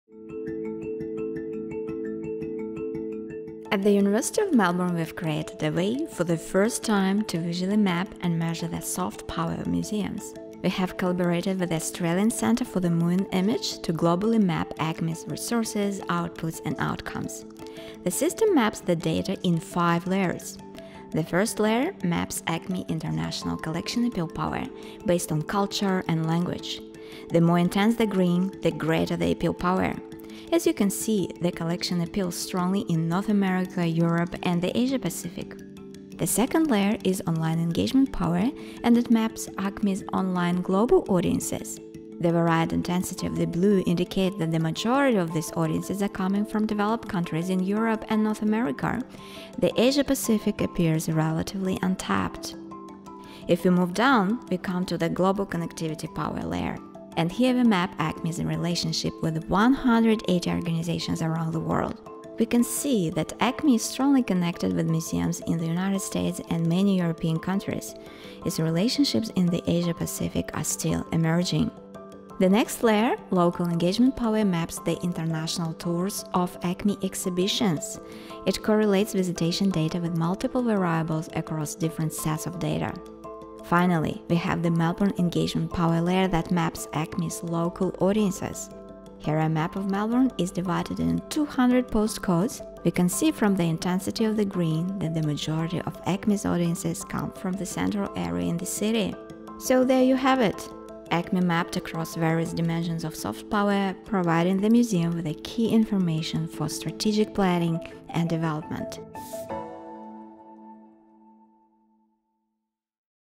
Mapping Museum Soft Power
Lead Chief Investigator, Dr Natalia Grincheva from the University Melbourne demonstrates a new online tool she has led the development of that can map the soft power of museums globally.
This pilot was created in collaboration with the Australian Centre for the Moving Image (ACMI).